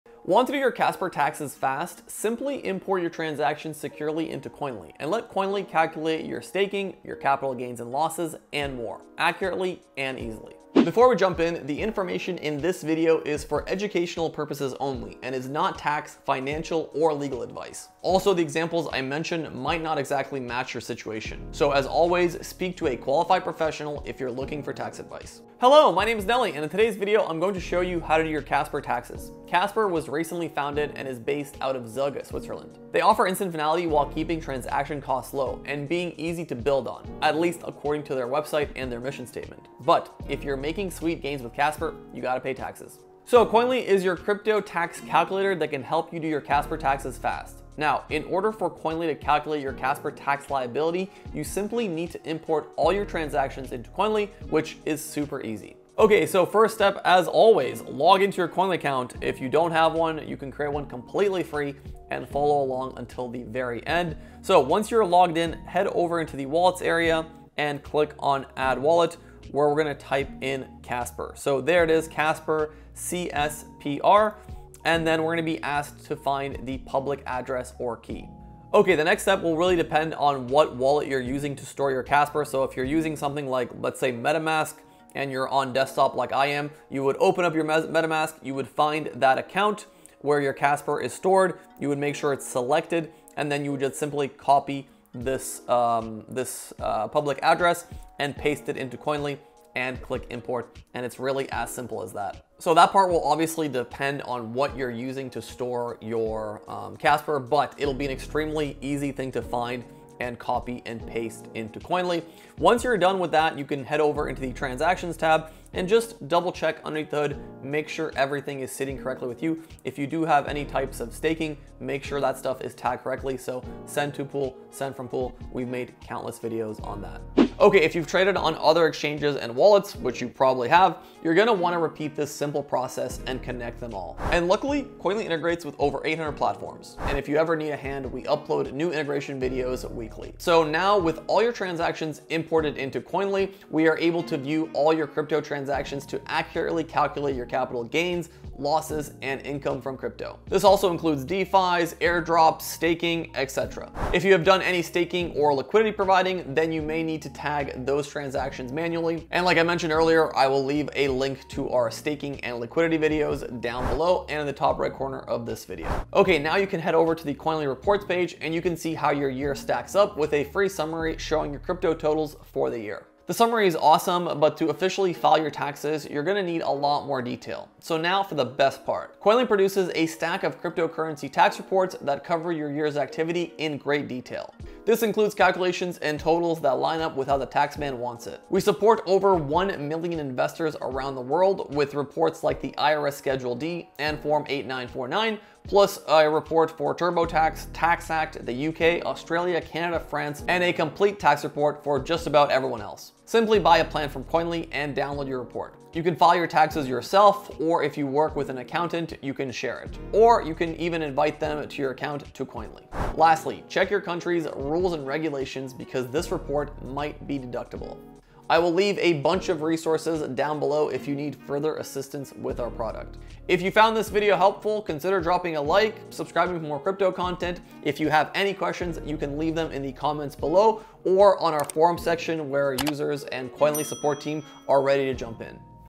How To Do Your Casper Taxes FAST With Koinly
Trading with Casper ? Not sure how to do your crypto tax? Worried about the IRS or your country’s tax authority? Relax, we've got you! Watch and learn with Koinly.

Using Casper and wondering how to do your Casper crypto taxes before the IRS, HMRC, CRA, or the ATO tax deadline? It’s really easy! Today I’ll show you have to quickly import your Wirex transactions into Koinly -  by importing different CSV files into Koinly. Koinly then calculates your cryptocurrency taxes and generates a fully compliant tax report that you file with yourself, or send to your crypto tax accountant. Let’s get your crypto tax sorted!

Want more info? Check out our step-by-step guide on how to get your Casper crypto tax done with Koinly